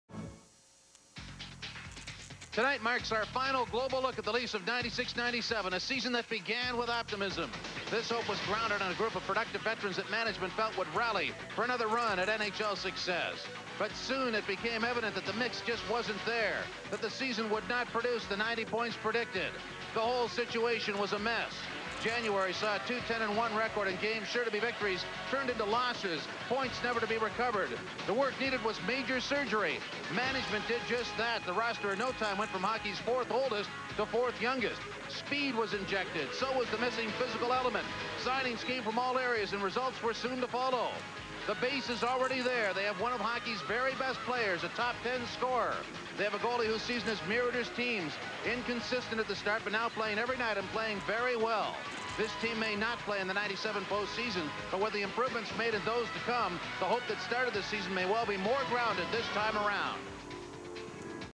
Toronto Maple Leafs intro 1997 "The whole situation was a mess"
This is a game intro for the last game on Global for the 1997 season. The season started with high hopes but this is the year that saw Gilmour and Muller traded away near the deadline. Jim Tatti sums it up as "the whole situation was a mess"

Lots of hope for the future though.